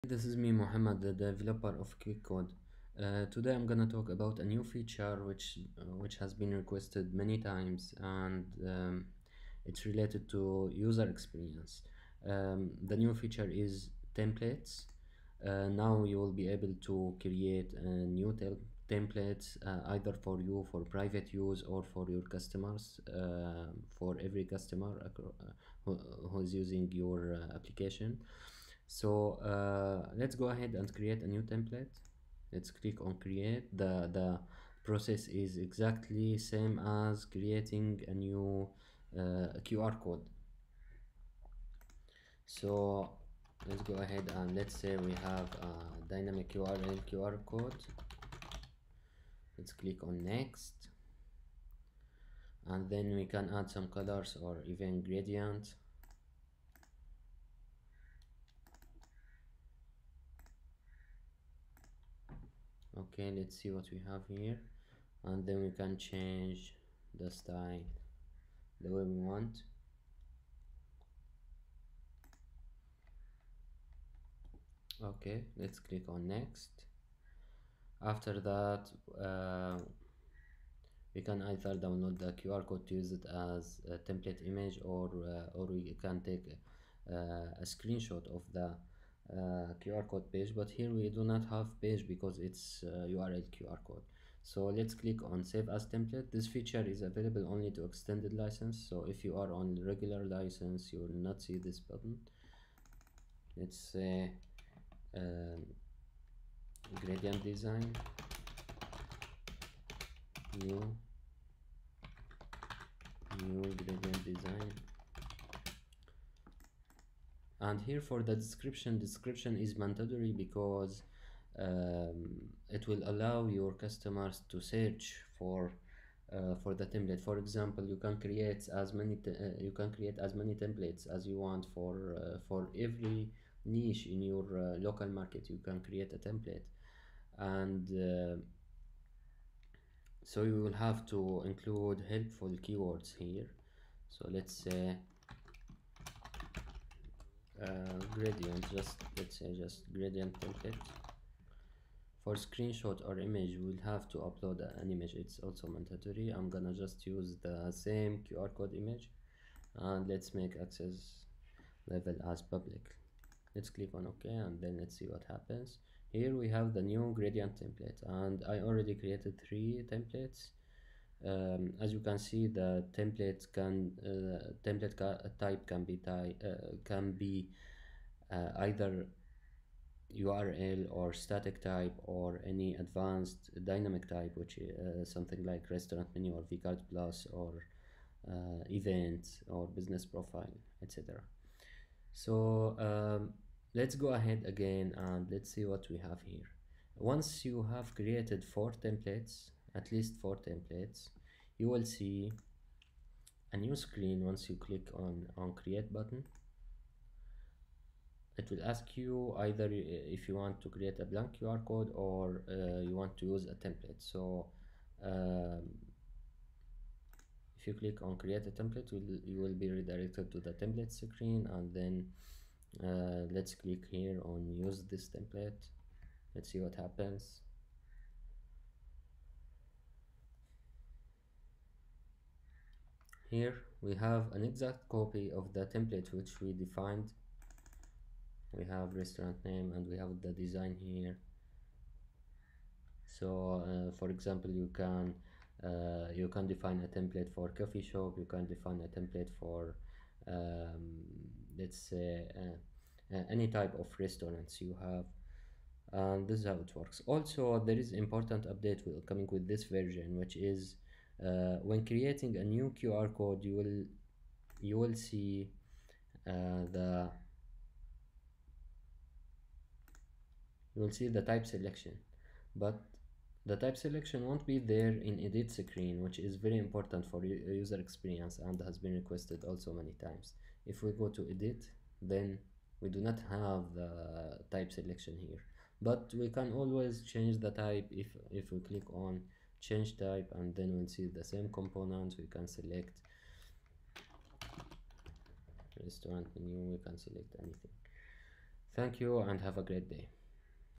QR Code Templates
With this feature you will be able to create unlimited QR code templates for you and your customers.